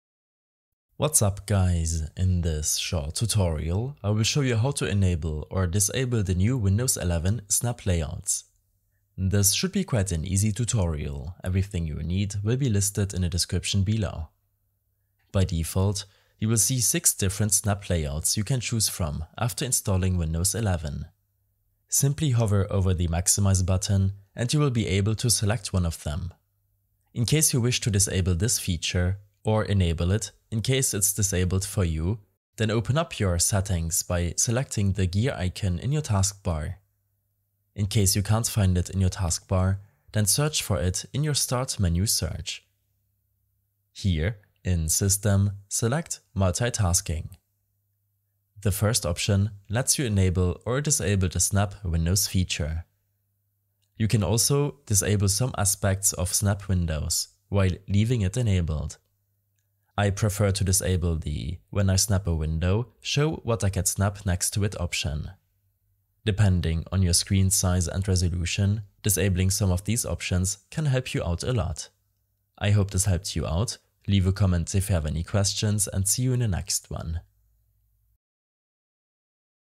Windows 11 - How To Enable & Disable Snap Layouts
A short tutorial on how to enable or disable the new Windows 11 snap layouts.